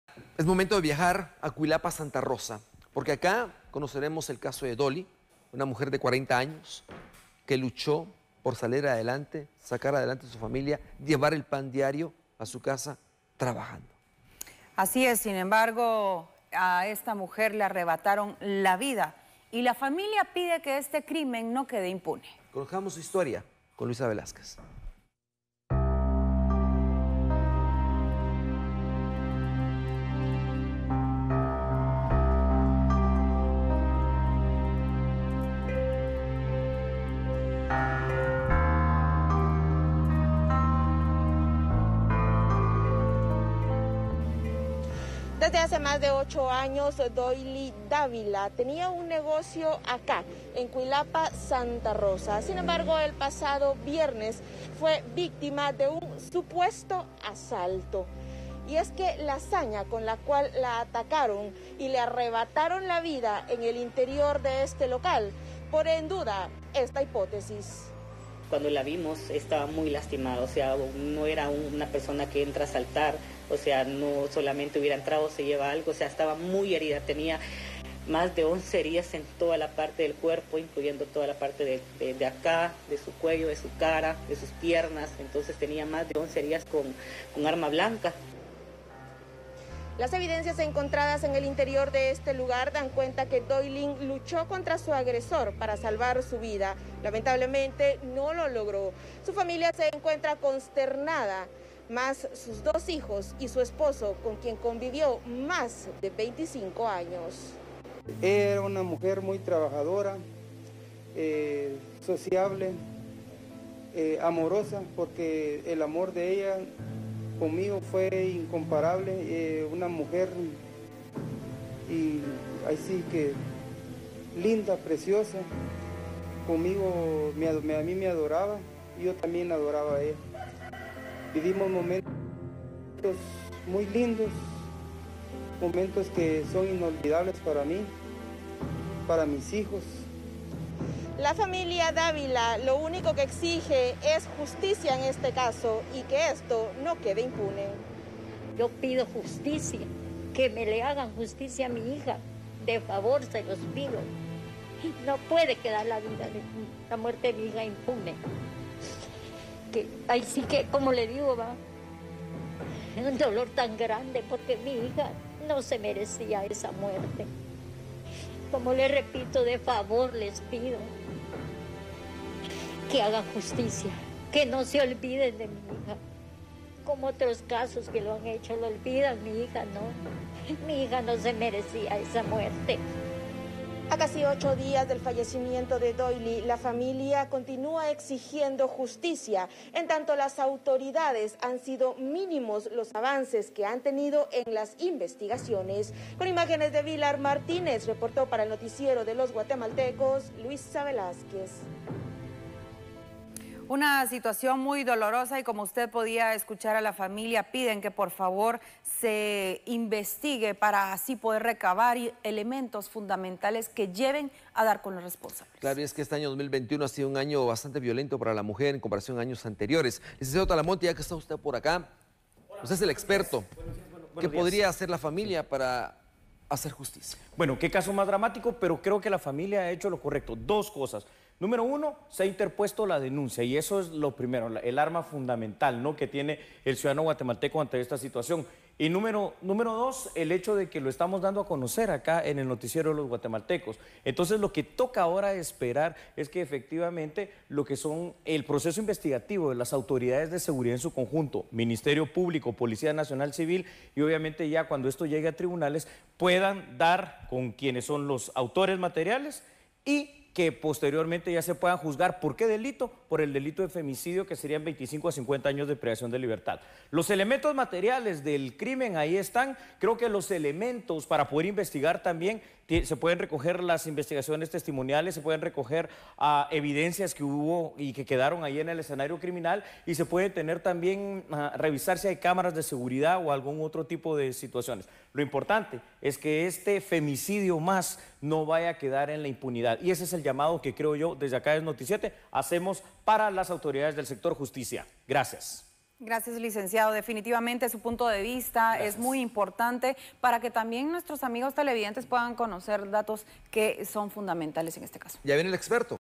Familiares piden justicia por asesinato de Doyly
Piden a las autoridades a que den con el responsable del crimen

Fue victima de un supuesto asalto en el interior de su local Familiares piden justicia por asesinato de Doyly Visita nuestro sitio